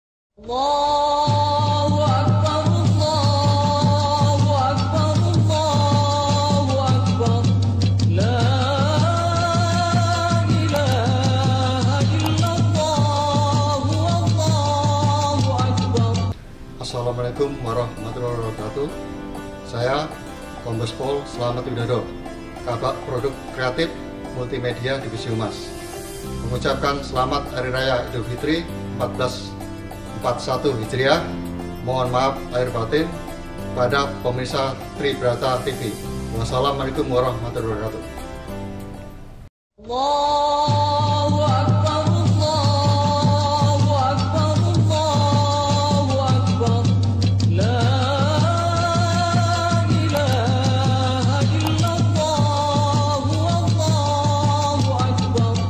KOMBES POL SLAMET WIDODO l KABAG PK MULTIMEDIA DIVHUMAS POLRI l - GREETING RAMADHAN 1441 H
Ucapan Kabag Pk Multimedia Divhumas POLRi Kombes Pol Slamet Widodo. Idul Fitri 1441 H